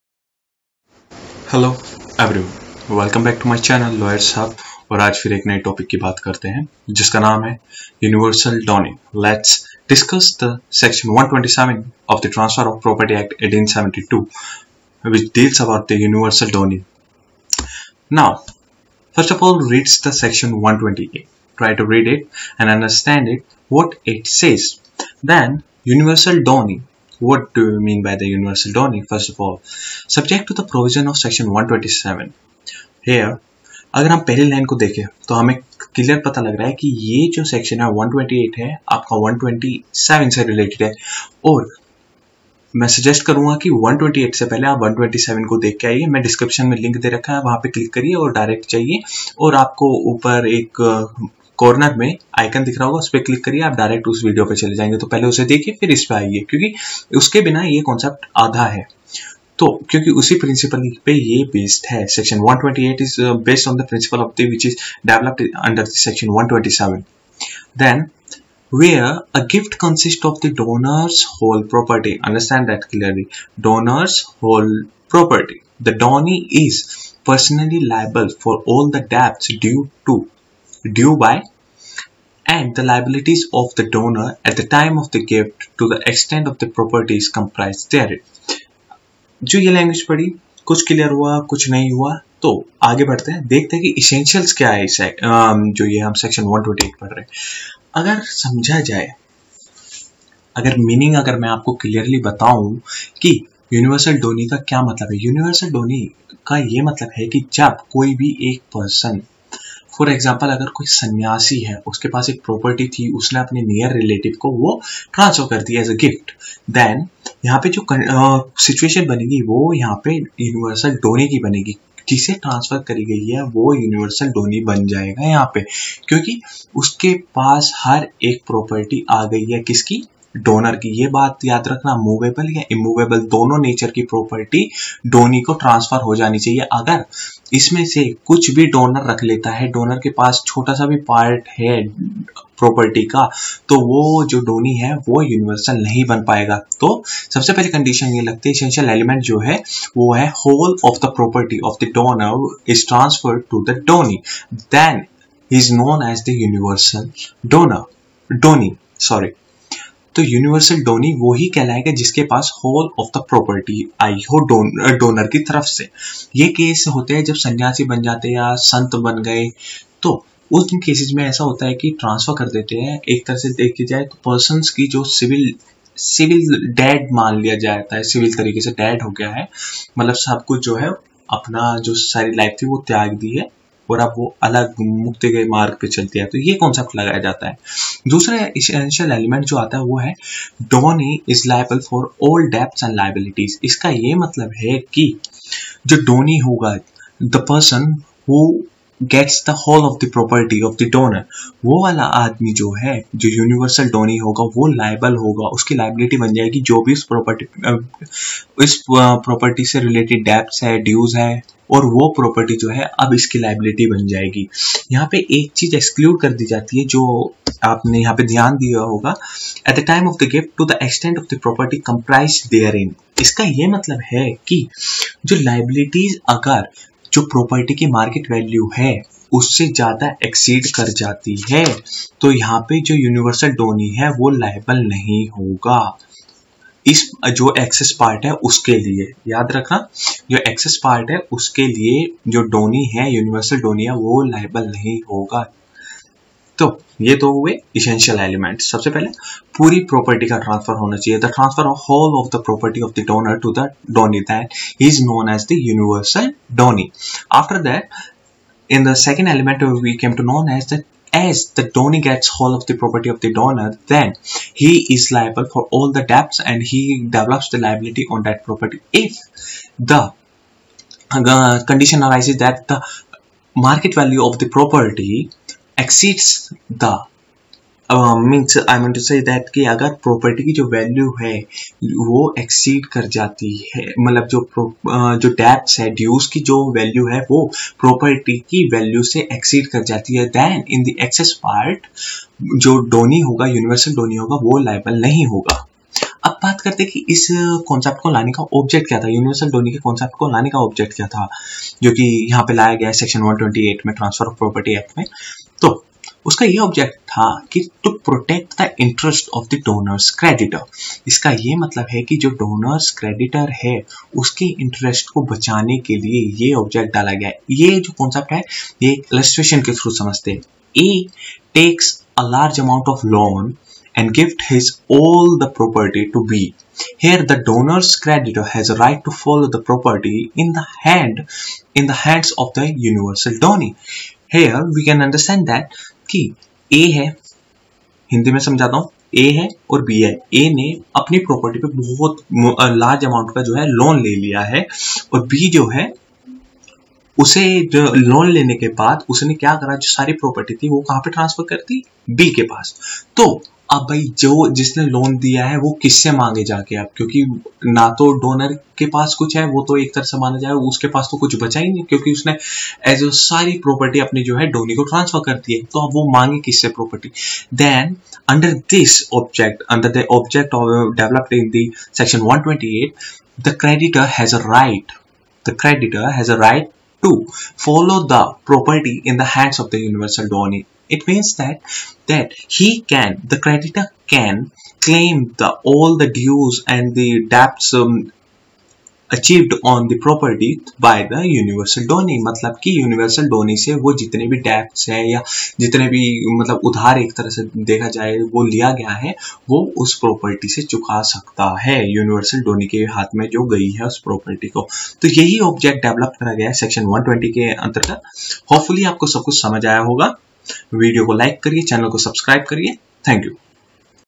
Universal Donee||section 127 transfer of property act,1872
in this video we are going to deal with the section 127 transfer of property act,1872 which  talks about the concept of universal donee
sec 127
object of the section
meaning